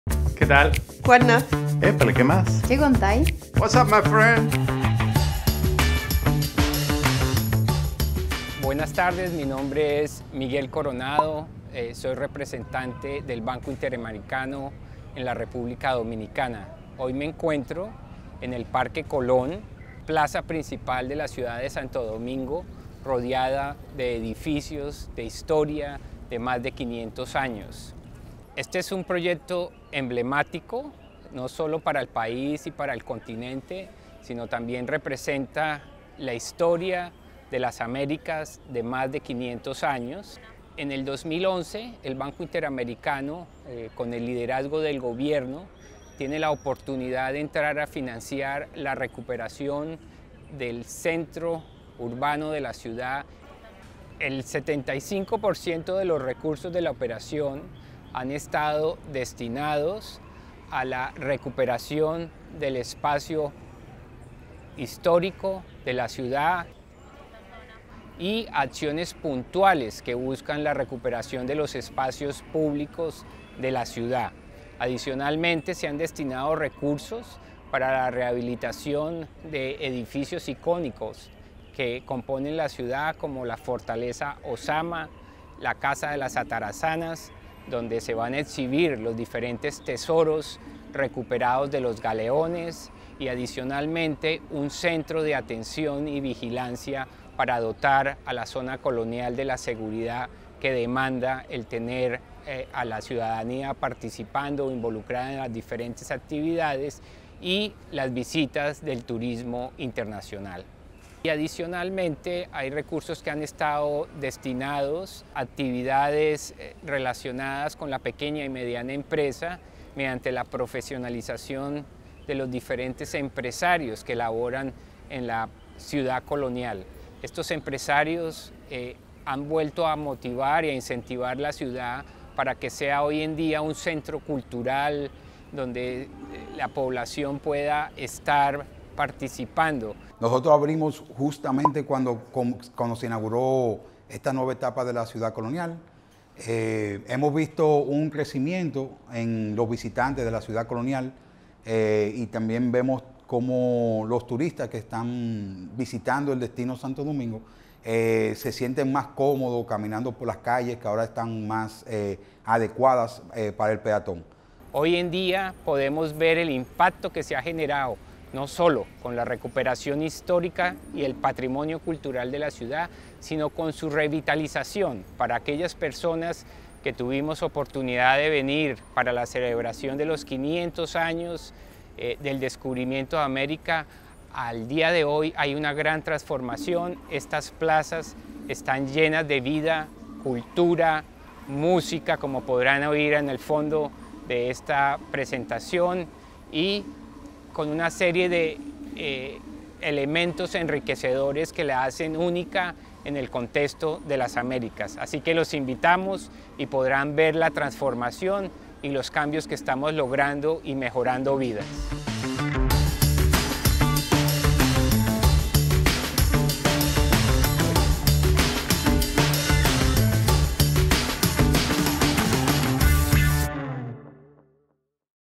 ¿Puede la recuperación de un casco histórico transformar una ciudad? El caso de Santo Domingo
La Ciudad Colonial de Santo Domingo es el primer asentamiento europeo permanente en América, declarada Patrimonio de la Humanidad por UNESCO en 1990. Conoce cómo ayudamos a renovar el espacio urbano, revitalizar la zona colonial y recuperar el patrimonio de Santo Domingo. Miguel Coronado, Representante del BID en República Dominicana, nos cuenta la historia transformadora de este casco histórico latinoamericano.